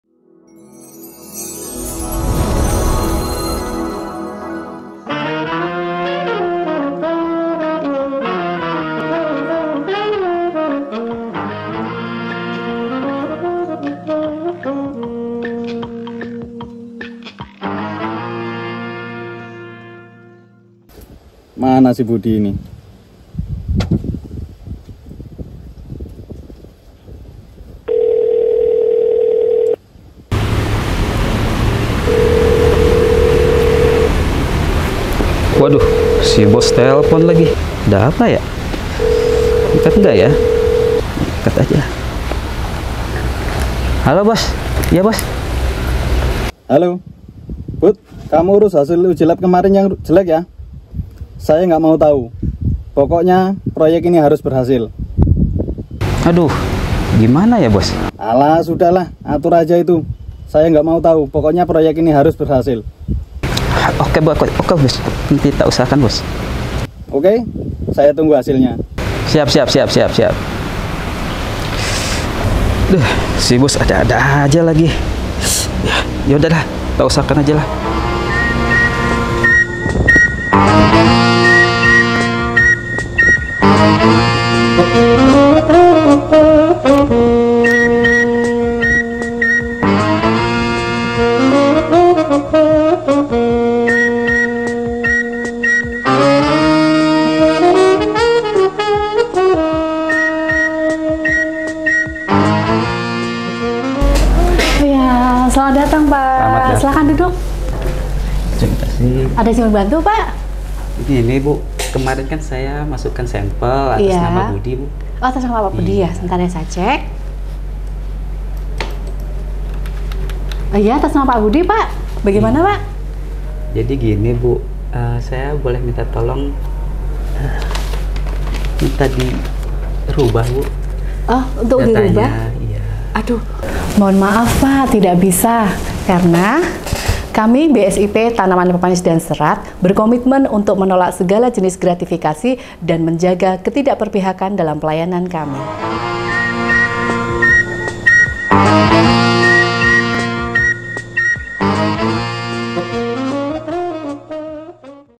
BSIP TAS : Stop Gratifikasi
Balai Pengujian Standar Instrumen Tanaman Pemanis dan Serat berkomitmen untuk bekerja dengan profesional tanpa gratifikasi.

Dukung kami, BSIP TAS menjaga integritas dengan TIDAK memberikan gratifikasi dalam bentuk apapun.

BSIP TAS menolak segala bentuk pemberian gratifikasi baik langsung maupun tak langsung yang berhubungan dengan jabatan.

Integritas adalah konsistensi berperilaku yang selaras dengan nilai, norma dan/atau etika organisasi, dan jujur dalam hubungan dengan atasan, rekan kerja, bawahan langsung, dan pemangku kepentingan, serta mendorong terciptanya budaya etika tinggi, bertanggungjawab atas tindakan atau keputusan beserta risiko yang menyertainya.

Pembangunan Integritas pegawai ASN adalah upaya untuk mewujudkan, memperkuat, dan mempertahankan nilai dasar, daya nalar dan keberanian moral ASN.